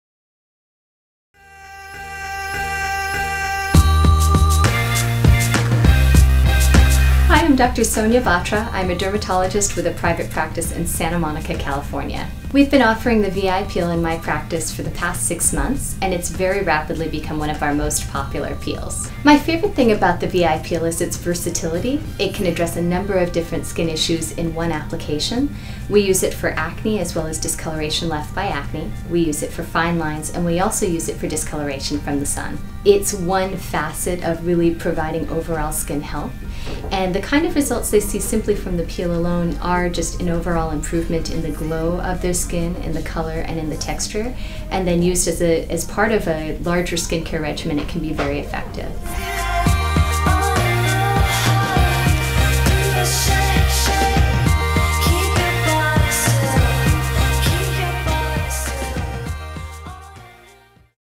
The Benefits of VI Peels - Winter Park, FL 32789
Improve the tone, texture and clarity of your skin by reducing acne, age spots, freckles, and pigmentation. Experience an increase in collagen for firmer, more youthful skin. Questions?